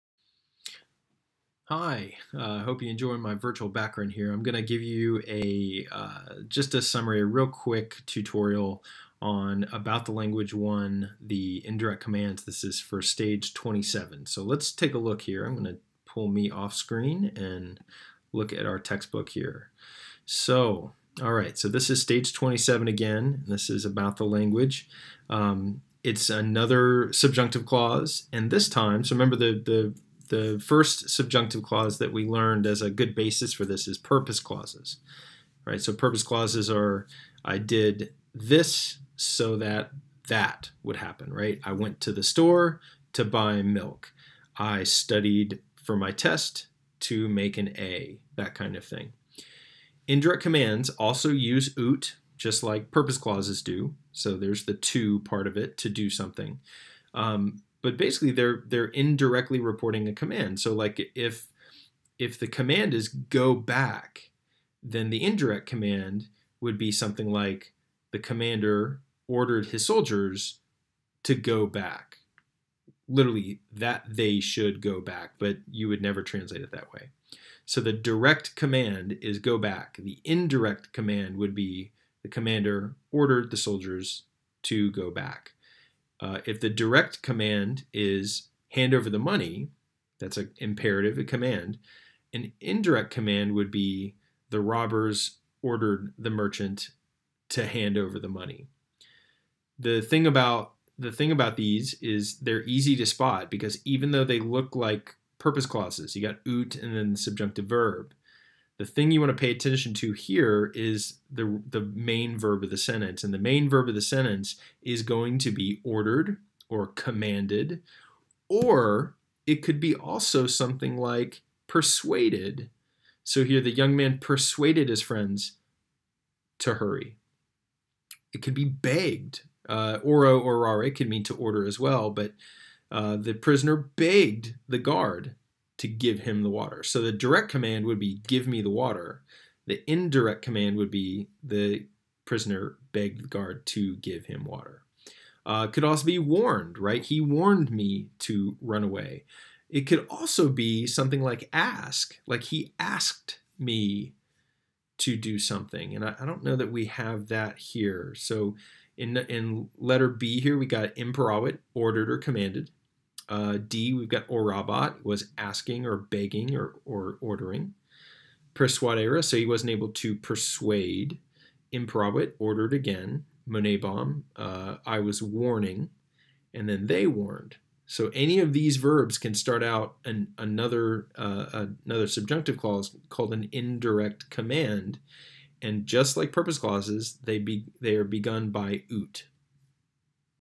Stage 27 ATL I Indirect Commands
A brief tutorial video on indirect commands, from About the Language 1 in Stage 27 of the Cambridge Latin Course.